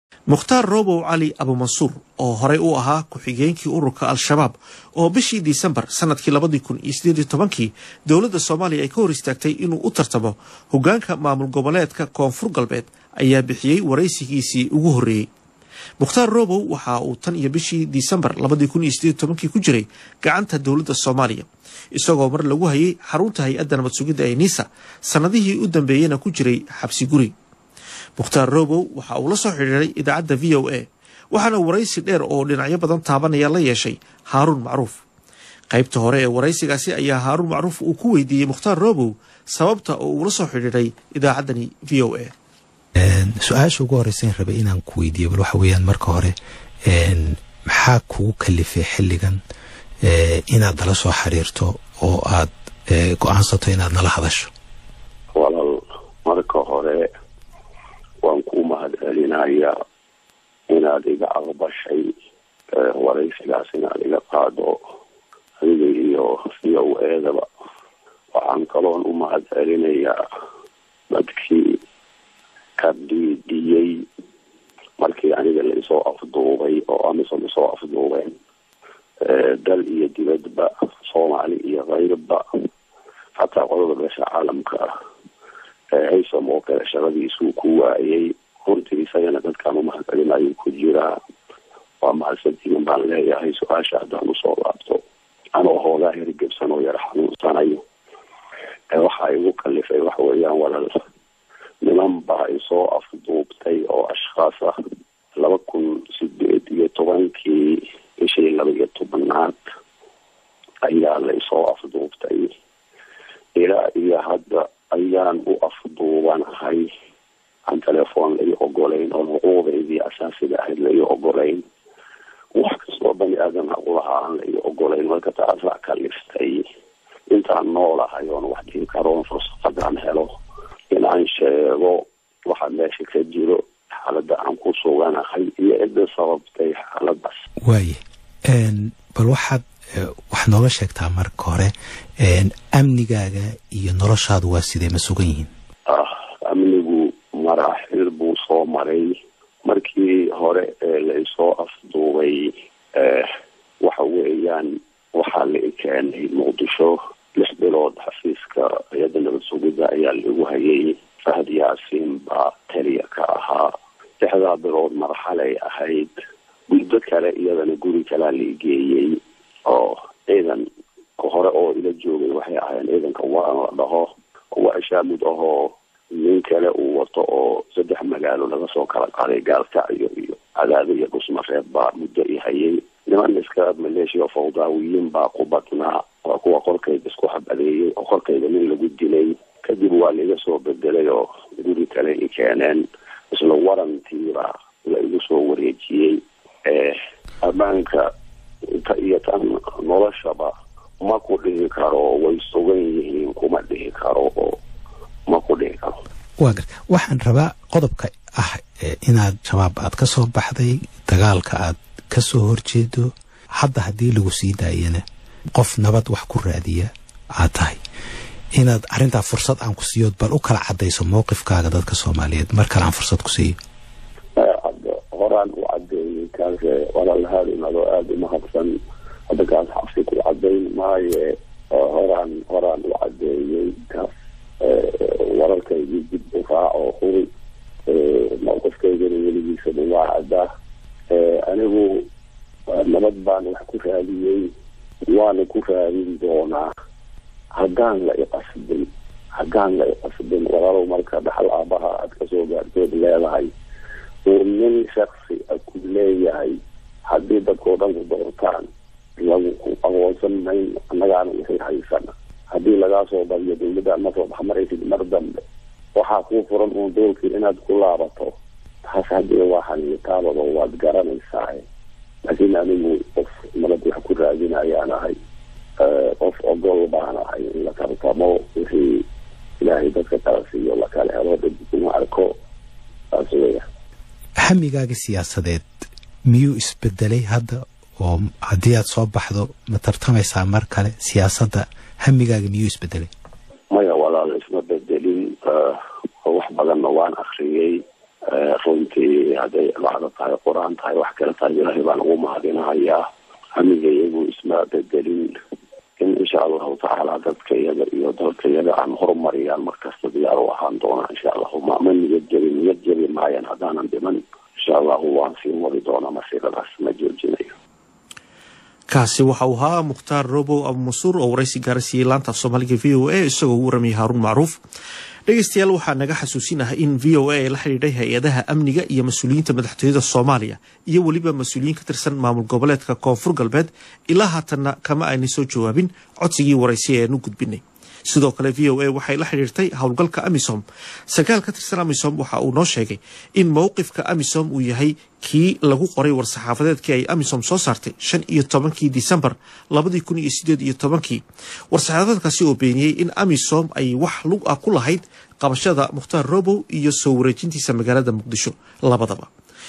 Abuu Mansuur oo markii ugu horeysay ka hadlay xarigiisa
Mukhtaar Roobow Cali (Abuu Mansuur) oo tan iyo 13-kii bishii December ee sanadkii 2018 u xiran Dowladda Federaalka ayaa markii ugu horeysay ka hadlay xarigiisa.